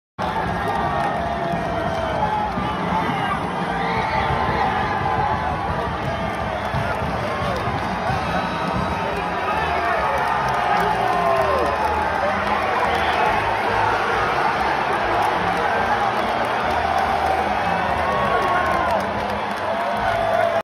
Eric Musselman of Arkansas removes his shirt after an upset victory over Kansas
Arkansas head coach Eric Musselman couldn't keep it together.